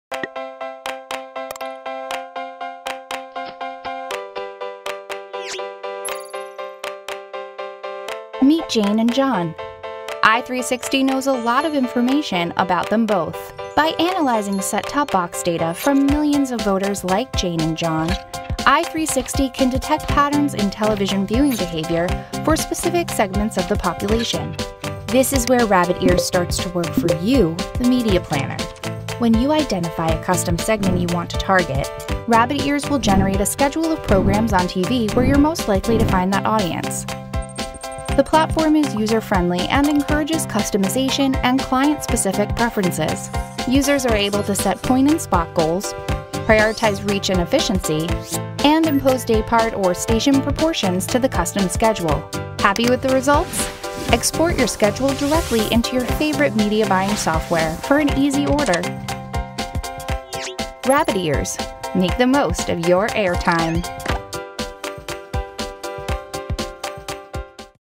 Introducing Rabbit Ears - Make The Most Of Your Airtime
By leveraging i360 custom political audiences matched to the historical viewership data from a collection of Set-Top Boxes (STBs), Rabbit Ears helps you identify which programs, dayparts and networks your target audience is likely to watch allowing you to build custom audience-informed schedules. Because there is no substitute for the media planner, all inputs are fully customizable allowing users the flexibility to set their own preferences. With RabbitEars, you can easily set your own point and spot goals, prioritize reach or efficiency, and set daypart or station proportions to create a schedule that is truly one of a kind. And, built on a “bring your own data” concept, the tool allows users to import any data they have and export their schedules to the Media Buying Software Platforms of their choice. Don’t have your data? Not to worry – i360’s premium audience data is also available and seamlessly integrated with the platform.